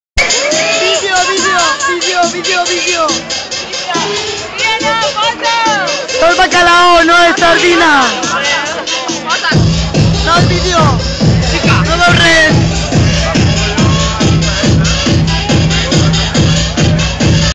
Açò és Bakalao, no és Sardina.mp4
A la discomòbil de El Lloc, a Pego, la pinyata'10!!